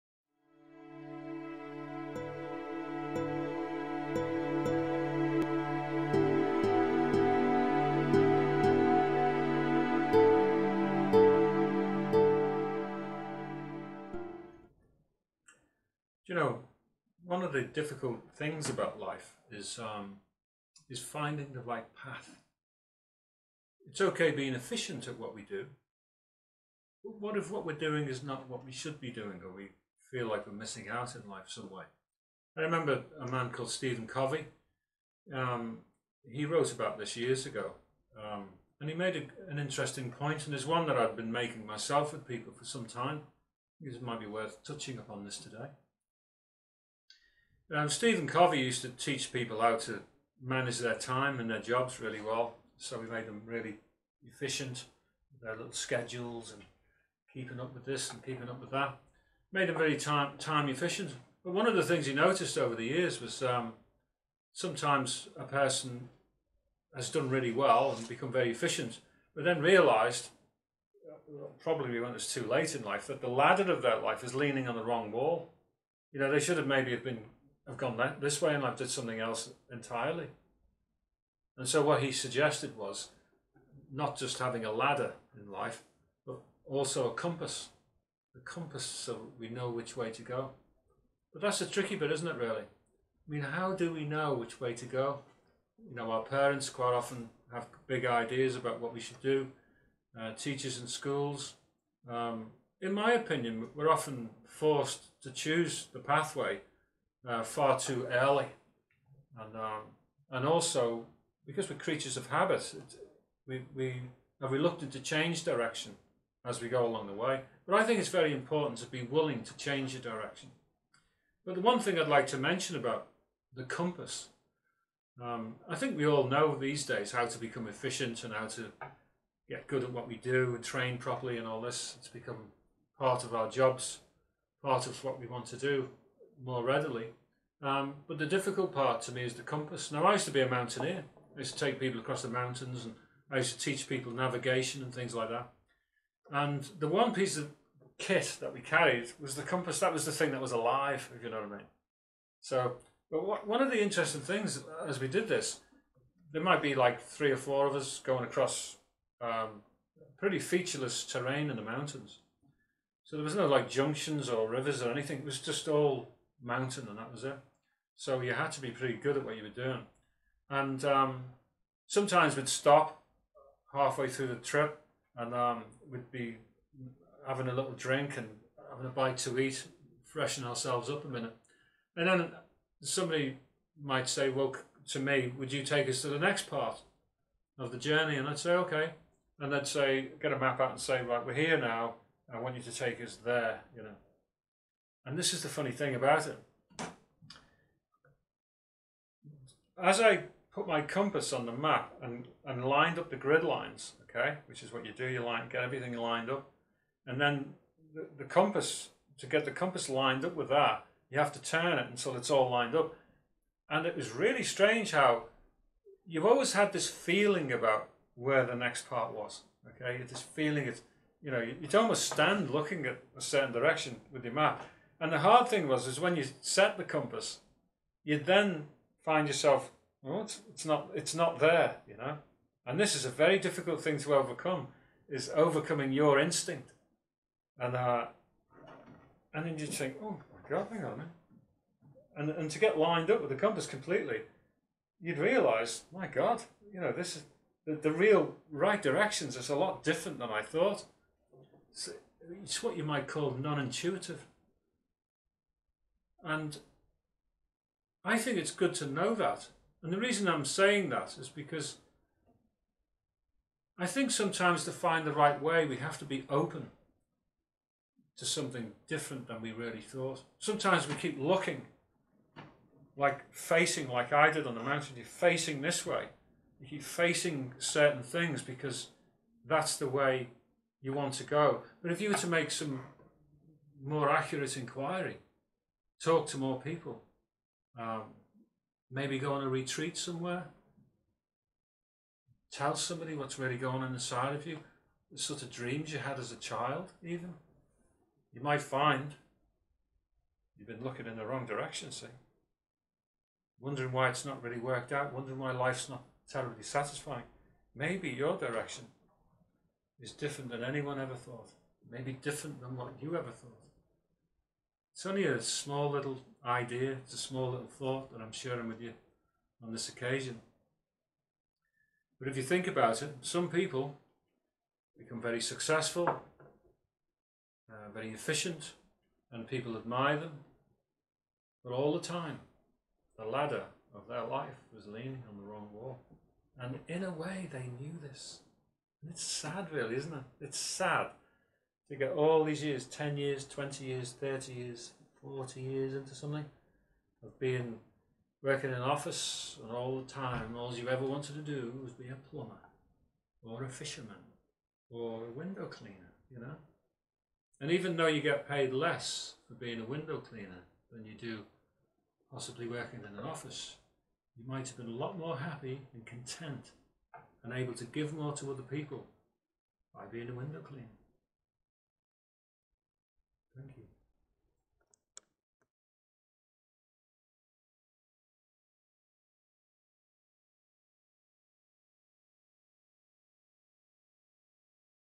Soul Talk Series | Finding The Right Path
How do we know which way to go in life? It can be a struggle to find the right path. If you are disheartened and feel you are not really fulfilled by your daily life and journey - then this talk is for you...
Author of "Another Kind Of Knowing" & the bestseller "Within These Streets"